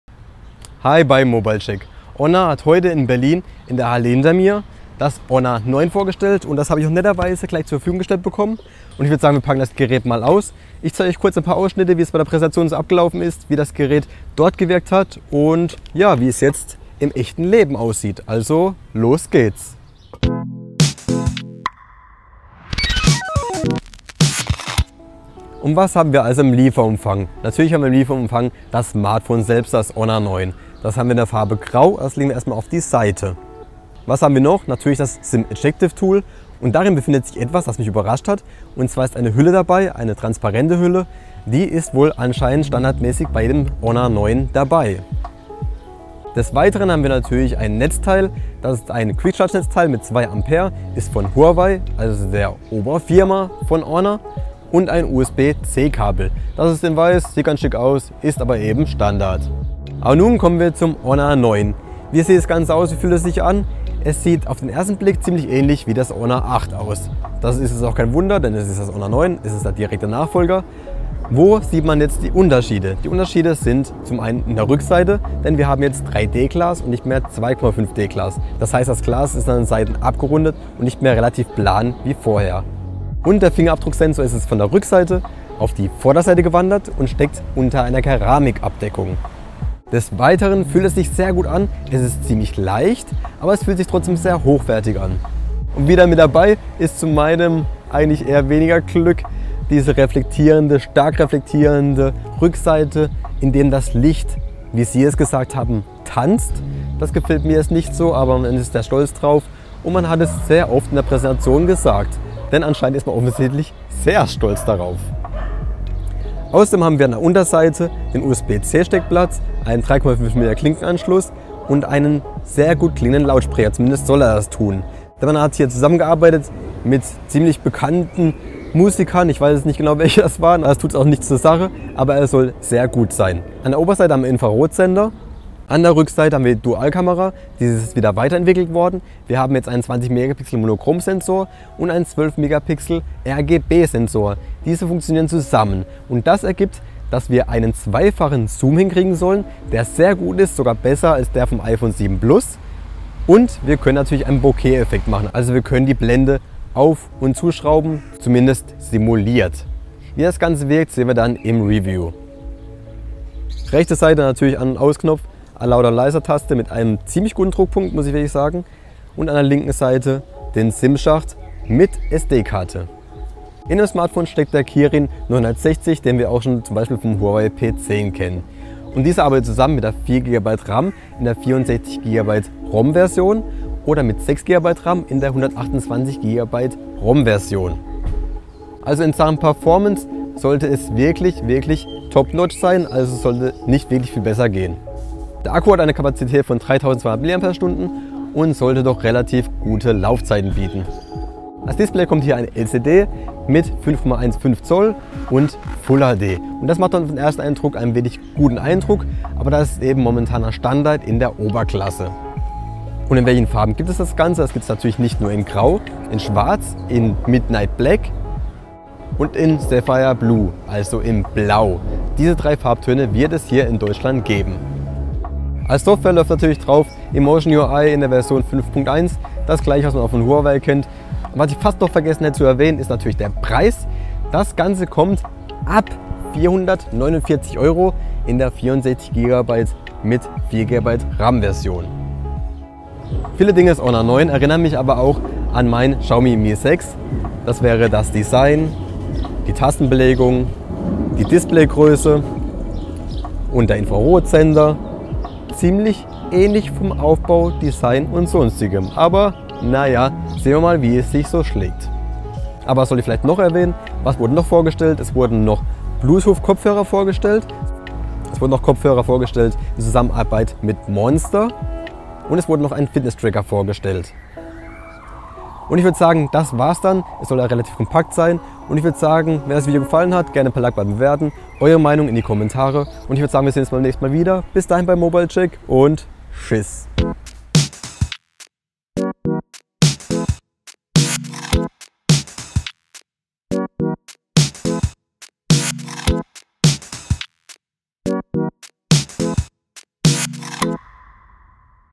Honor 9 ! Unboxing & erster Eindruck
Das Honor 9 wurde am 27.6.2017 in Berlin für den europäischen Markt vorgestellt und löst nun den erfolgreichen Vorgänger Honor 8 ab. Ich war dort um es für euch mal genauer anzuschauen.